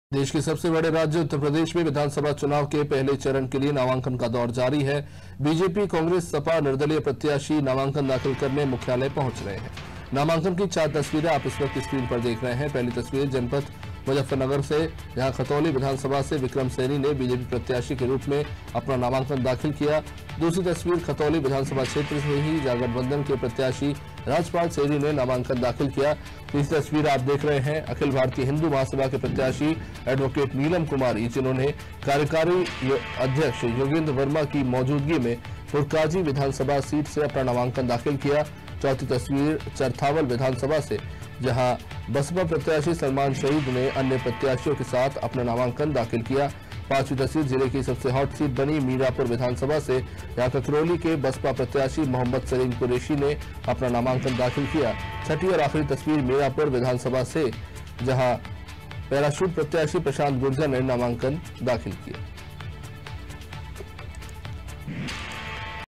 प्रत्यासियो ने दाखिल किया नामांकन | UP ELECTION NEWS | Hindi news | Latest news | Anaadi TV
देश के सबसे बड़े राज्य उत्तर प्रदेश में विधानसभा चुनाव के पहले चरण के लिए नामांकन का दौर जारी है...बीजेपी, कांग्रेस सपा और निर्दलीय प्रत्याशी नामांकन दाखिल करने मुख्यालय पहुंच रहे हैं...नामांकन की चार तस्वीरें आपकी टीवी स्क्रीम पर मौजूद हैं...पहली तस्वीर जनपद मुजफ्फरनगर से है...जहां खतौली विधानसभा से विक्रम सैनी ने बीजेपी प्रत्याशी के रूप में नामांकन दाखिल किया...दूसरी तस्वीर भी खतौली विधानसभा क्षेत्र से है...जहां गठबंधन के प्रत्याशी राजपाल सैनी ने नामांकन दाखिल किया..तीसरी तस्वीर में आप देख रहे हैं...अखिल भारतीय हिंदू महासभा के प्रत्याशी एडवोकेट नीलम कुमारी को...जिन्होने कार्यकारी अध्यक्ष योगेंद्र वर्मा की मौजूदगी में पुरकाजी विधानसभा सीट से नामांकन पत्र दाखिल किया...चौथी तस्वीर चरथावल विधानसभा से है...जहां बसपा प्रत्याशी सलमान शईद ने अन्य प्रत्याशियों के साथ नामांकन दाखिल किया...पांचवीं तस्वीर जिले की सबसे हॉट सीट बनी मीरापुर विधानसभा से है...जहां ककरौली के बसपा प्रत्याशी मोहम्मद सालीम कुरैशी ने अपना नामांकन दाखिल किया...छठी तस्वीर मीरापुर विधानसभा से है...जहां बीजेपी के पैराशूट प्रत्याशी प्रशांत गुर्जर ने नामांकन दाखिल किया...